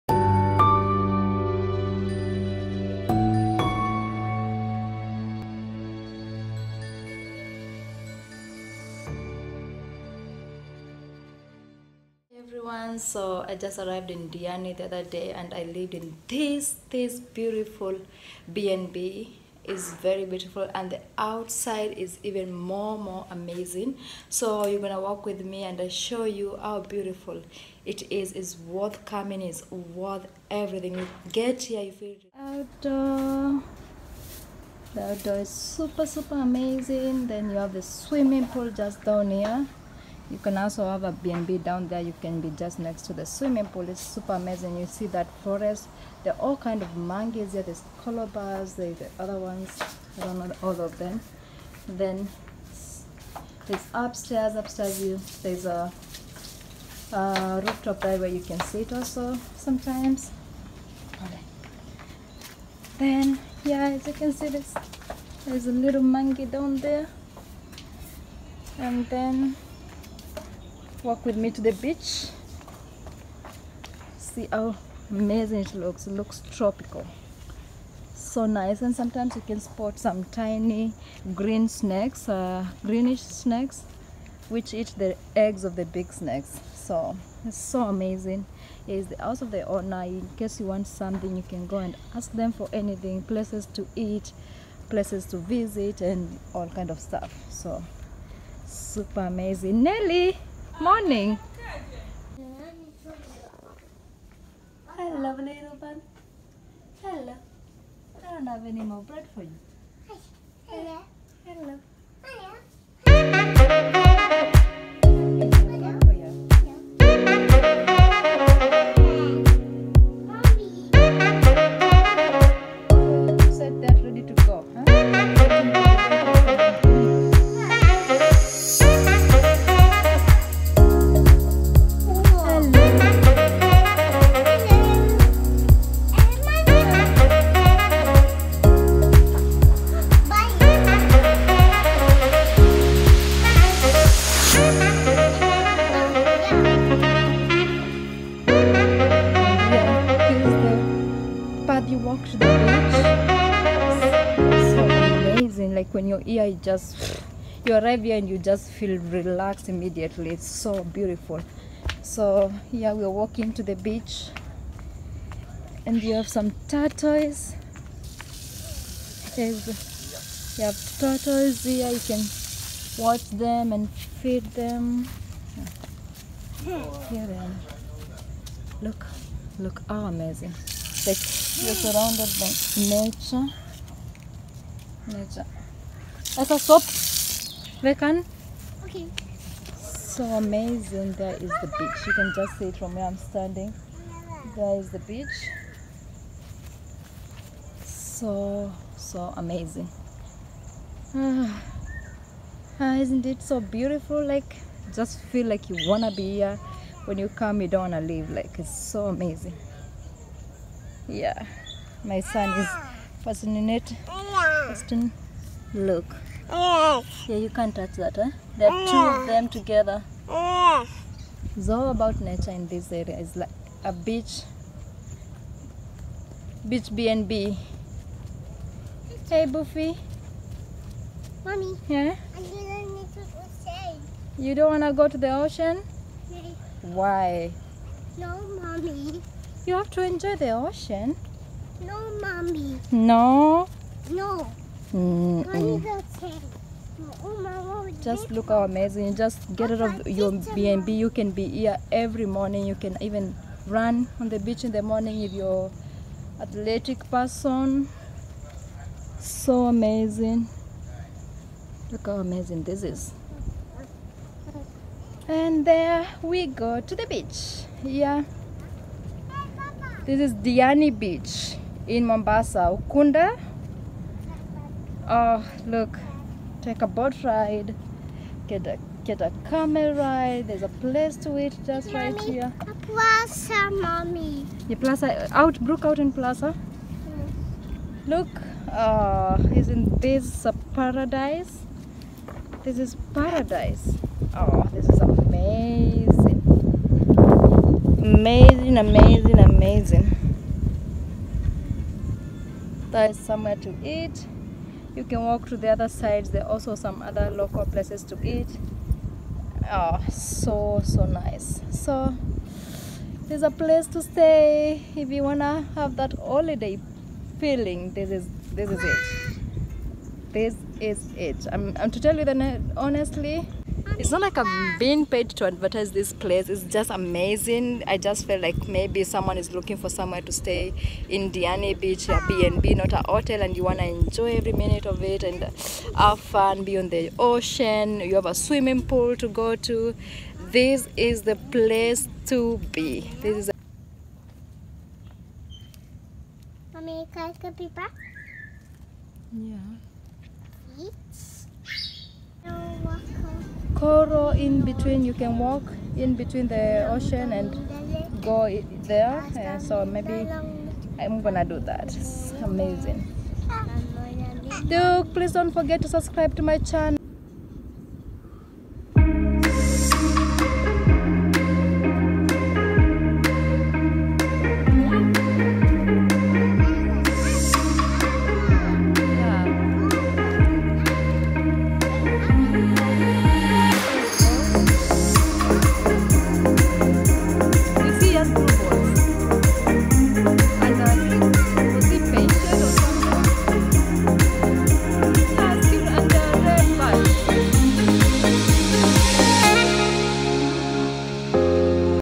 Best Airbnb Diani beach mombasa {Diani airbnb tour}
Best Airbnb diani beach mombasa

video content
00:00 intro
00:12 arrived
00:34 outdoor
00:44 forest
00:51 monkeys
01:14 see the sorrounding
01:28 owner
02:50 turtules
03:14 the beach
03:31 my sun